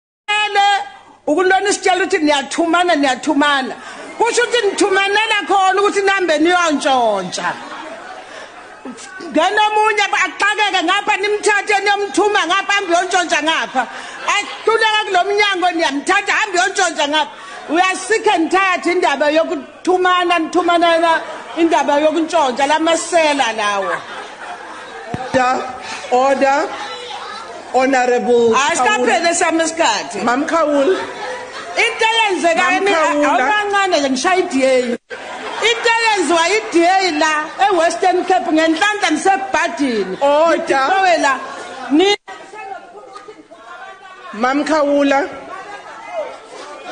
"Nithumana Ukuyotjontja" - Mam Khawula to ANC
One of our own, Mam Khawula here is debating on corrupt and criminals who were once again put in power in order for them to go and steal some more. The looters themselves! Aluta